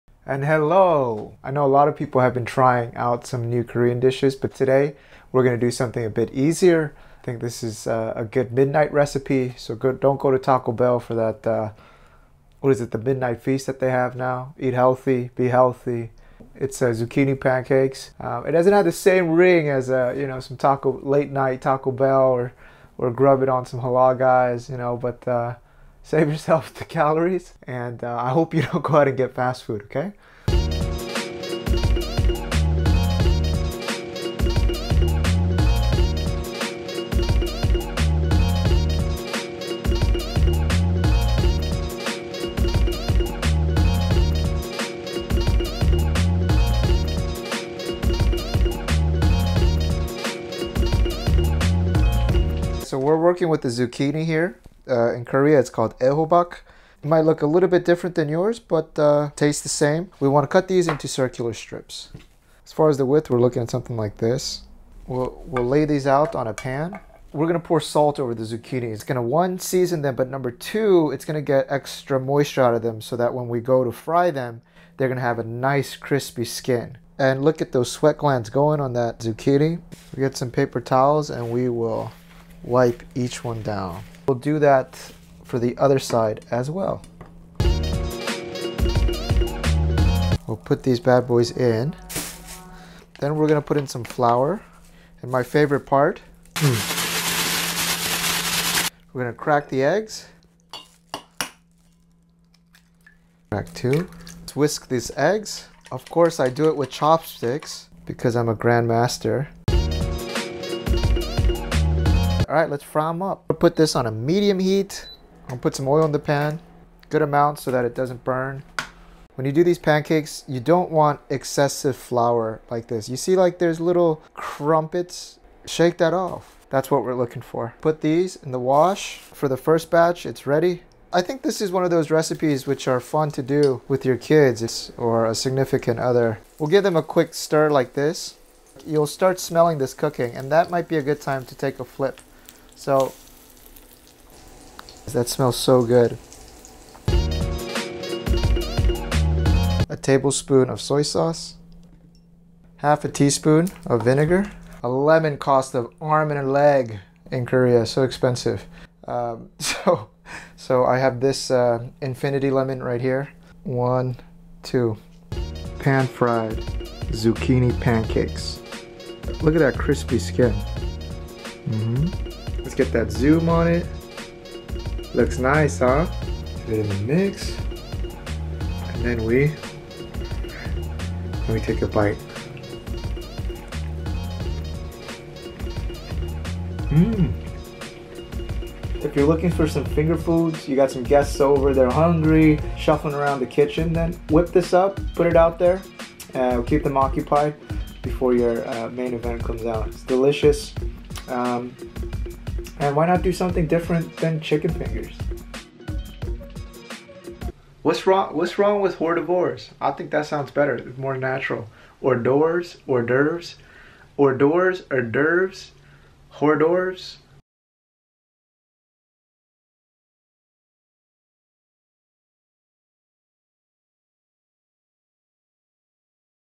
How to make Zucchini Tots (Banchan) | 애호박전
Daniel shows you how to make Zucchini Tots - 애호박전. It is very simple to make for yourself. All you need is a fresh zucchini, some flour and eggs. Whip these up for guests before dinner or make them together with the kids on a weekend. Simple, healthy and delicious! Try it out at home.

*Recipe by FutureNeighbor